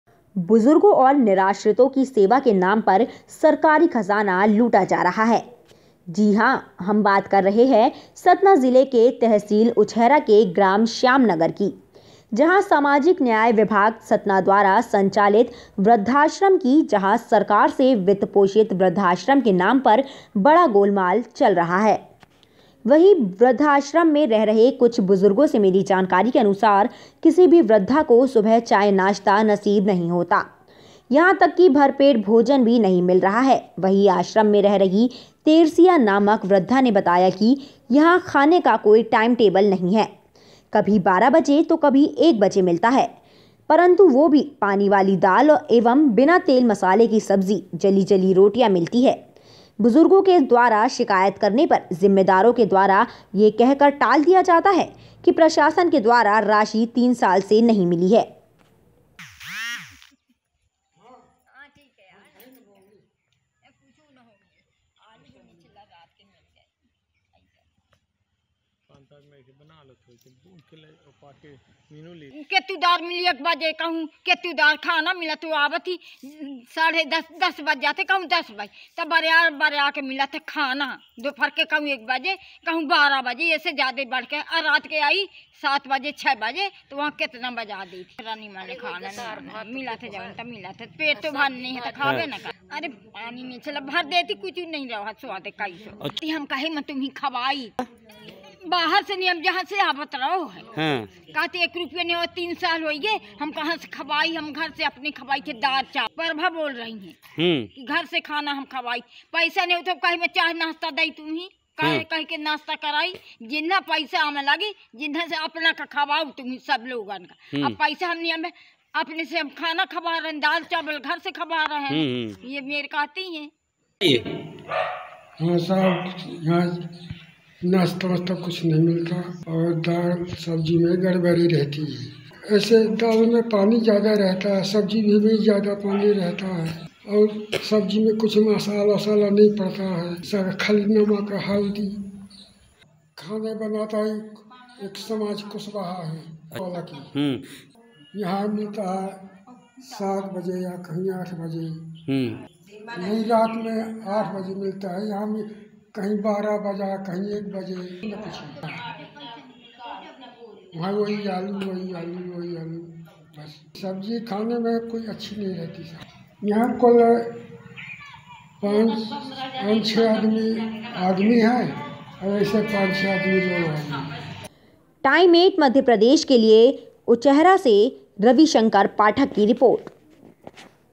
सेवा के नाम पर खजाने की लूट। TIME8 MADHYA PRADESH
उंचेहरा। बुजुर्गों और निराश्रितों की सेवा के नाम पर सरकारी खजाना लूटा जा रहा है । हम बात कर रहे हैं सतना जिले के तहसील उंचेहरा के ग्राम श्यामनगर की जहां सामाजिक न्याय विभाग सतना द्वारा संचालित बृद्धाश्रम की जहाँ सरकार से वित्त पोषित वृद्धाश्रम के नाम पर बड़ा गोलमाल चल रहा है ।